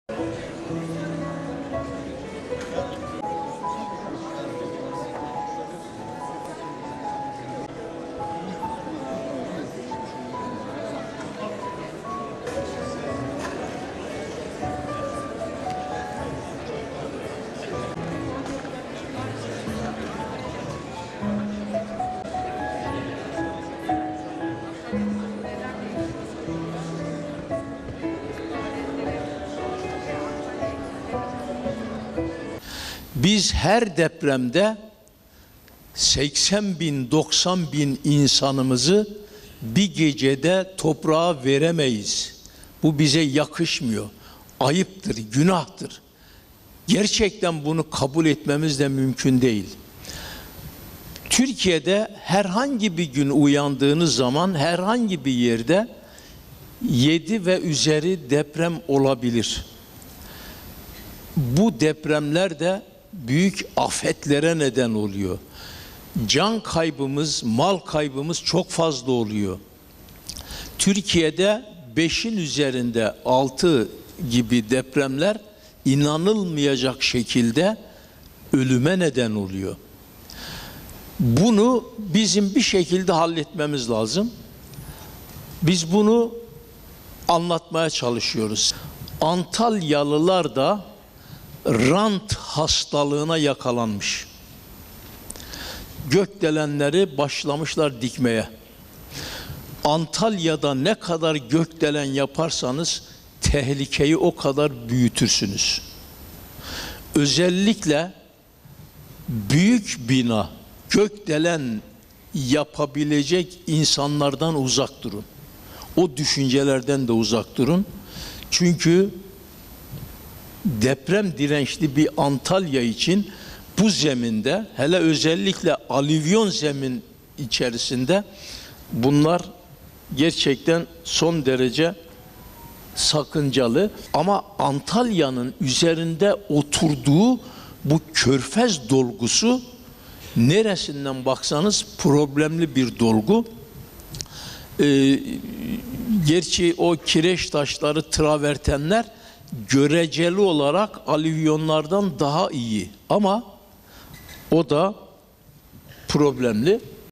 ANTALYA Prof. Dr. Görür: Türkiye'de herhangi bir yerde, sabah uyandığımızda 7 ve üzeri deprem ola...
BİLİM Akademisi üyesi, Yer Bilimci Prof. Dr. Naci Görür, "Her depremde 80-90 bin insanı bir gecede toprağa veremeyiz. Bu bize yakışmıyor. Türkiye'de herhangi bir yerde, sabah uyandığımızda 7 ve üzeri deprem olabilir. Türkiye'de 5'in üzerinde depremler ölüme neden oluyor. Bizim bunu halletmemiz lazım" dedi.

Prof. Dr. Naci Görür, Antalya Büyükşehir Belediyesi tarafından düzenlenen 'Anttalks' etkinliği kapsamında AKM'de 'Deprem Dirençli Antalya' adlı söyleşiye katıldı. Prof. Dr. Görür, 13,6 milyon sene önce Türkiye'nin levha hareketlerinin, bugünün hareketlerini yapacak şekilde oluştuğunu ve ülkedeki büyük fayların deprem ürettiğini söyledi. Türkiye'nin depremlerden kurtulamayacağını ve depremlerin sona ermeyeceğine dikkati çeken Prof. Dr. Görür, deprem dirençli yerleşim alanlarının oluşturulmasının önemini vurguladı. Prof. Dr. Görür, "Her depremde 80-90 bin insanı bir gecede toprağa veremeyiz. Bu bize yakışmıyor. Türkiye'de herhangi bir yerde, sabah uyandığımızda 7 ve üzeri deprem olabilir. Bu depremler de büyük afetlere neden oluyor. Can mal kaybımız fazla oluyor. Türkiye'de 5'in üzerinde depremler ölüme neden oluyor. Bizim bunu halletmemiz lazım. Bunu anlatmaya çalışıyoruz" diye konuştu. 6 Şubat'taki Kahramanmaraş merkezli depremlerin ardından yakın zamanda Japonya'da 7.6 büyüklüğünde deprem olduğunu ve 132 kişinin tesadüfen öldüğünü anlatan Prof. Dr. Görür, benzer depremin Türkiye'de olması halinde on binlerce kişinin ölebileceğinden bahsetti.

'GÖKDELENLERİ DİKMEYE BAŞLAMIŞLAR'
Antalya'nın jeolojisi hakkında bilgi veren Prof. Dr. Görür, Antalya'nın genç çökeller üzerine kurulduğunu söyledi. Antalya'nın büyük kısmının zemininin iyi ya da mükemmel olmadığına dikkati çeken Prof. Dr. Görür, falez ve traverten gibi karasal nitelikli kireç taşlar ile alüvyon dolguların üzerinde kentin büyük bölümünün yer aldığını belirtti. Kentin doğusundaki zeminin alüvyon dolgusundan oluştuğunu aktaran Prof. Dr. Görür, "Antalyalılar rant hastalığına yakalanmış. Gökdelenleri dikmeye başlamışlar. Antalya'da ne kadar gökdelen yaparsanız, tehlikeyi büyütürsünüz. Özellikle büyük bina, gökdelen yapabilecek insanlardan uzak durun. O düşüncelerden de uzak durun. Deprem dirençli Antalya için özellikle alüvyon dolgu, zemin için son derece sakıncalı. Antalya'nın üzerinde oturduğu körfez dolgusu, problemli bir dolgu. Kireç taşları ve travertenler göreceli olarak alüvyonlardan daha iyi ama o da problemli" dedi.

'ANTALYA AKTİF FAYLARLA ÇEVRİLİ'
Antalya'yı tehdit eden fay sistemleri hakkında bilgi veren Prof. Dr. Görür, şunları söyledi:
"Akşehir zonu, Isparta-Burdur zonu ile Göller Yöresi'nin bulunduğu yerler düşey atımlı fayların olduğu bölge. Antalya Körfez doğu, batı ve kuzeyden aktif faylarla çevrili. Antalya fay zonlarına 100 kilometreden uzakta değil, 50-60 kilometre uzakta. Bu fay kuşaklarında olacak depremler, Antalya'yı önemli etkiler. Doğrultu atımlı faylarda 100 kilometrelik çap içerisindeki yöreler depremden etkilenir. Antalya'nın güneyinde Akdeniz içerisinde Kıbrıs, Girit bölgesinde Helen yayı dediğimiz dalma batma zonu var. Üç büyük tektonik hat Antalya'yı tehdit etmekte. Güneyden dalma batma zonu büyük deprem üretebilir. Girit yakınlarında 8 büyüklüğünün üzerinde deprem üretmiş, her an 7'nin üzerinde deprem üretebilir. Bu tehlike denizden gelir. Antalya'yı tehdit eden fay söz konusu. Akşehir ve Fethiye Burdur zonu deprem ürettiği zaman Antalya ciddi şekilde etkilenebilir."

'ANTALYA'NIN DOĞUSU DAHA ÇOK HASAR ALIR'
Yerel yönetimlere çağrıda bulunan Prof. Dr. Görür, Antalya'yı tehdit eden fayların incelenmesi, risk analizi ile risk azaltma çalışması yapılması gerektiğini ifade etti. Antalya'ya yapılacak altyapı çalışmaları ve yatırımların deprem dalgalarının kentin yapısına etkisinin belirlendikten sonra gerçekleştirilmesi gerektiğine değinen Prof. Dr. Görür, "Ciddi deprem olsa Antalya'nın doğusu daha çok hasar görür. Alüvyonun üzerinde olduğu için sıvılaşma da olur. O bölgeye gökdelenler dikiliyor. Batısında traverten veya kireç taşlarının üzerinde olan binalar yüksek olmamak kaydıyla biraz daha dirençli olabilirler. Alüvyonun bulunduğu yerlerde şansınız yok. Deprem anında alüvyonlar içlerinde su tuttuğu için çökellerdeki su basıncı yukarı iletiyor. Yukarı iletince evlerin direnci azalıyor. Bina suyun içerisinde gibi hareket etmeye başlıyor ya dönüyor ya da batıyor" diye konuştu.

5 MİLYON TON TNT PATLAMASINA BENZER ENERJİ
Depremin büyüklüğü ve şiddeti kavramları arasındaki farkı da anlatan Prof. Dr. Görür, Kahramanmaraş merkezli depremlerde 5 milyon ton TNT patlamasına benzer enerjinin açığa çıktığını söyledi. Yerel yönetimlerin kentleri depreme hazırlamaya yönelik çalışmaları hızlandırması gerektiğini anlatan Prof. Dr. Görür, altyapı çalışmalarında betonarme boru kullanılmasını eleştirdi. Betonarme boruların kırılgan yapısından dolayı depremden etkilendiğini a...